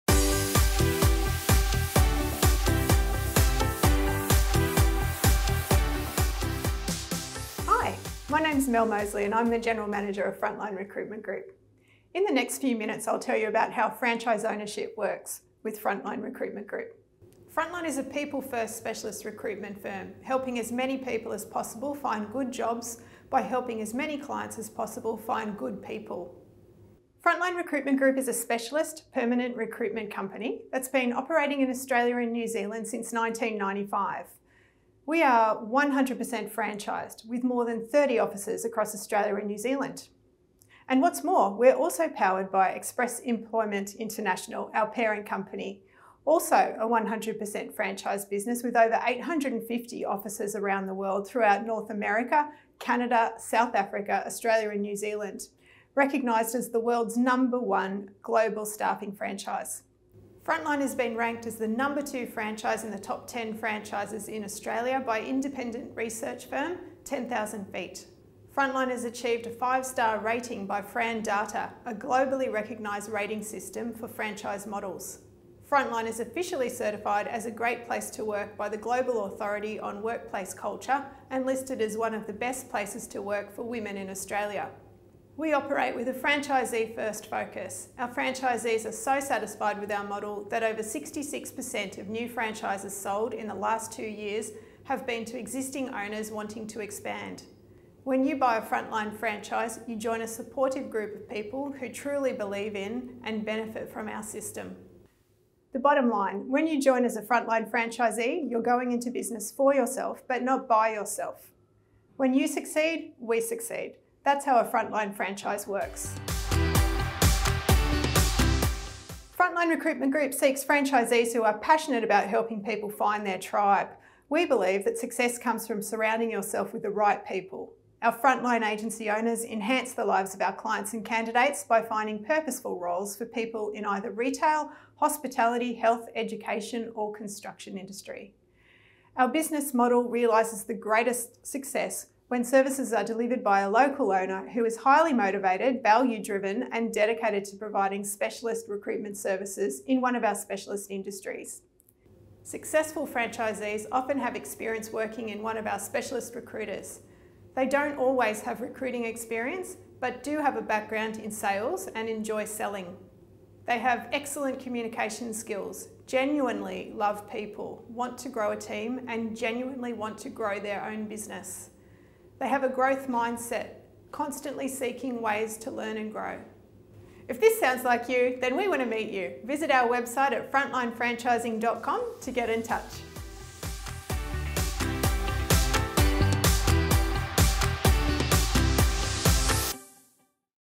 How Franchise Ownership Works With Frontline Recruitment Group (Short Version)
When you own a Frontline Recruitment franchise, we help you find your people.

Frontline Recruitment Group was founded IN 1995 and is now part of The Express Employment Professionals Group, a global staffing franchise employing over 552,000 people across 860+ franchised locations.

Frontline Franchisees are supported by a proven system for starting and running a successful recruitment business, allowing individuals with no previous recruitment experience to build a business in recruitment in their area of specialisation.